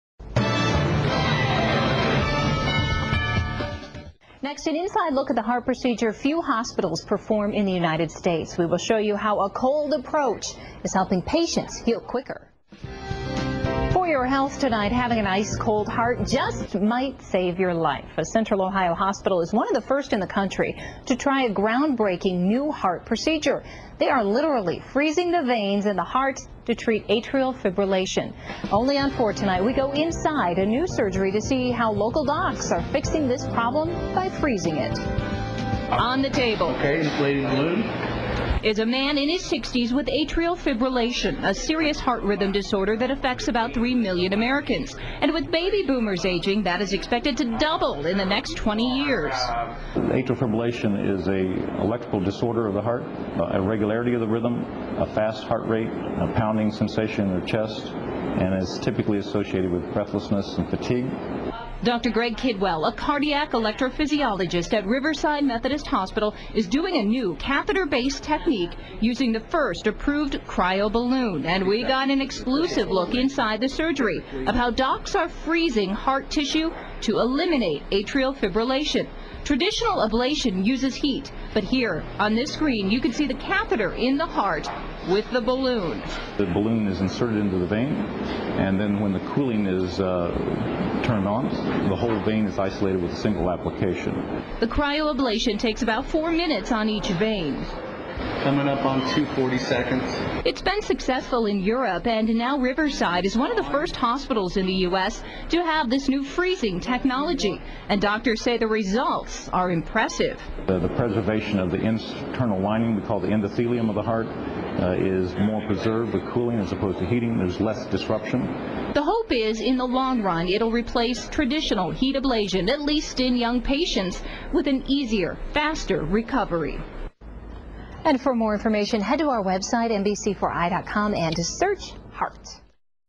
Riverside Methodist Hospital's New Heart Arrhythmia Treatment
Riverside Methodist Hospitals recently became one of the first medical centers in the United States to obtain new technology to treat one type of atrial fibrillation, a serious heart rhythm disorder that affects about three million Americans.

The catheter-based technique uses the first approved cryoballoon to safely and effectively freeze and isolate the cardiac tissue responsible for the initiation of paroxysmal atrial fibrillation (PAF), with the goal of eliminating the rhythm disorder at its source.

"This is an exciting innovation that provides us with a more efficient approach over existing methods to correct this problem in patients who do not respond to medication," said Dr. Gregory A. Kidwell, MD, a cardiac electrophysiologist at Riverside and Dublin Methodist Hospital. "With this technology, we can more easily restore the heart's natural rhythm and return patients to normal activities soon after treatment."

Atrial fibrillation is characterized by irregular and often rapid rhythms in the upper heart chambers (atria), resulting in typical symptoms of palpitation, breathlessness and fatigue.  If left untreated, atrial fibrillation increases the risk of stroke and heart failure. PAF is a type of atrial fibrillation that stops and starts on its own. However, the arrhythmia can transition into more persistent or permanent forms - particularly if not managed properly.

The new catheter ablation procedure at Riverside Methodist prevents unwanted electrical currents from traveling from the pulmonary veins (large blood vessels that carry blood from the lungs to the left atrium of the heart) and spreading to the atria (the upper chambers of the heart), which causes atrial fibrillation.

During the procedure, an electrophysiologist threads a catheter from the groin to the left atrium of the heart and then positions it at the opening of the pulmonary vein. A balloon is then inflated, closing off the vein completely. It is filled with coolant, which creates a circular lesion or scarring that disables the unwanted electrical currents causing the abnormal heart rhythm.

"Complete isolation of the pulmonary veins is critical for a successful outcome in patients with PAF", Dr. Kidwell said. "Our early results with this tool have been impressive, with a 100 percent isolation of the targeted veins."

Riverside Methodist Hospital uses the Arctic Front Cardiac CryoAblation Catheter system by Medtronic for this procedure. It is the only cryoballoon catheter approved in the United States for the treatment of atrial fibrillation.

Results from the STOP AF (Sustained Treatment of Paroxysmal Atrial Fibrillation trial) were the basis for U.S. Food and Drug Administration approval late last year. They showed that 69.9 percent of those treated with the Arctic Front catheter had no atrial fibrillation after one year, compared to 7.3 percent on drug therapy. The device was also shown be safe, with just 3.1 percent experiencing procedure-related adverse events.